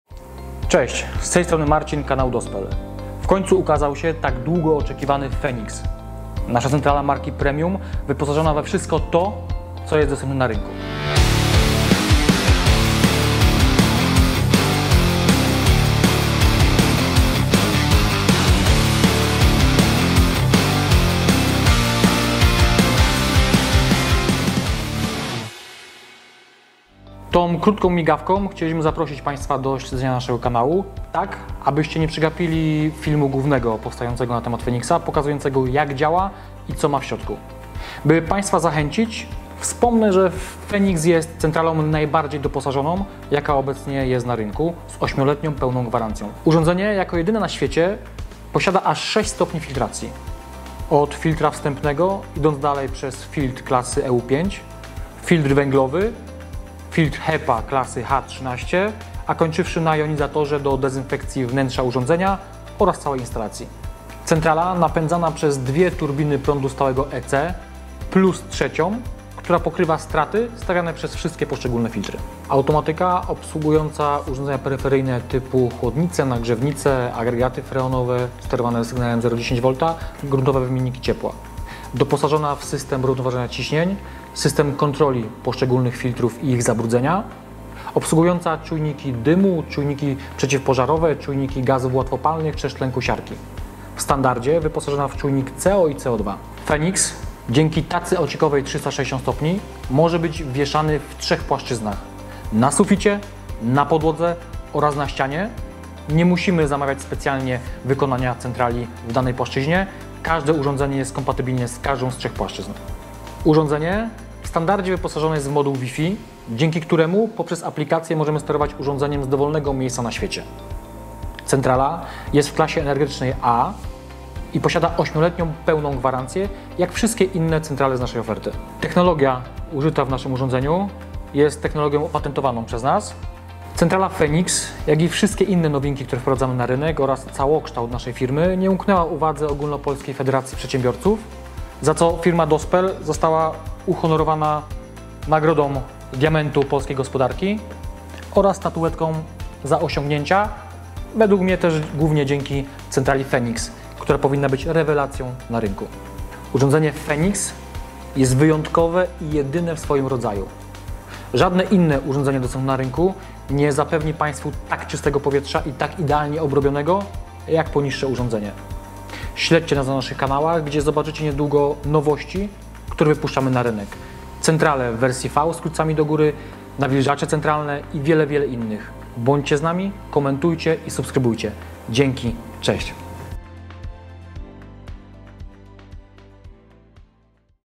FENIX NAJLEPSZY ECO REKUPERATOR NA ŚWIECIE | 6 stopni FILTRACJI | 98% ODZYSKU | KLASA A | BERLÜF
NAJWYŻSZY STOPIEŃ ODZYSKU CIEPŁA 98%

Dzięki zastosowaniu TRZECH WYMIENNIKÓW KRZYŻOWO-PRZECIWPRĄDOWYCH o łącznej powierzchni wymiany ciepła niespełna 25 m2, co czyni go numerem 1 w swojej klasie

3 ENERGOOSZCZĘDNE SILNIKI EC z płynną regulacją mocy, w zależności od  zapotrzebowania, co przekłada się na oszczędność energii.

NAJDŁUŻSZA 8-LETNIA GWARANCJA obejmująca wszystkie podzespoły centrali

NAJCICHSZY SYSTEM
Dzięki technologii skupiającej się na wyciszeniu samego źródła hałasu przy użyciu kilku autorskich metod, czego nie zastosowano w żadnej innej centrali. Całkowicie cicha praca pozwala na umiejscowienie urządzenia nawet w pomieszczeniach mieszkalnych

PODWÓJNA IZOLACJA CIEPLNA
Dzięki zastosowaniu profesjonalnej piany pur, stosowanej dotychczas w centralach przemysłowych w najwyższej klasie przepuszczalności

PEŁNE BEZPIECZEŃSTWO
Centrala reguluje poziom tlenu na podstawie pomiaru niezawodnych czujników stężenia  co, co2 i tym samym nie dopuszcza do niebezpiecznego dla życia poziomu co2 w pomieszczeniu.

OPATENTOWANY SYSTEM 3w1
Umożliwia montaż jednego urządzenia w trzech pozycjach: podwieszanej,  stojącej  i leżącej, co daje wiele możliwości instalatorom.

INTUICYJNY PANEL DOTYKOWY PLUM znany i ceniony przez użytkowników

NAJBARDZIEJ ROZBUDOWANA AUTOMATYKA obsługująca wszystkie urządzenia peryferyjne jakie mogą być użyte w instalacji

MODUŁ WIFI umożliwia sterowanie centralą z każdego zakątka świata poprzez aplikację

WERSJA ZE STALI NIERDZEWNEJ z atestem higienicznym i przeznaczeniem do pomieszczeń o podwyższonej sterylności

BY-PASS ABSOLUTNY gwarantuje maksymalne wykorzystanie do chłodzenia powietrza latem

PODWÓJNY SYSTEM ANTYZMROŻENIOWY
Realizowany poprzez modulację pracy silników w skrajnych przypadkach wspomagany grzałką inwerterową o mocy 300 W

50 Lat doświadczenia w produkcji wentylacji
21 Lat doświadczenia w produkcji rekuperacji
8 Lat gwarancji na wszystkie rekuperatory Dospel